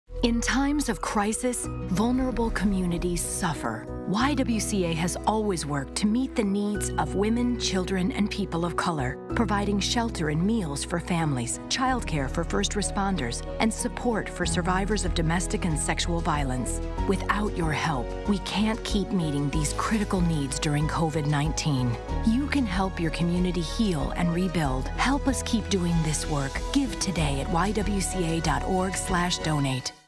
YWCA PSA: Meeting Critical Needs at a Critical Time
In times of crisis, vulnerable communities suffer. YWCA provides critical, live-saving needs to the most vulnerable populations every day, and we continue to do that work during the novel coronavirus crisis. But we can’t do it alone. Watch our PSA to learn how we’re helping communities heal and help us continue the work by donating today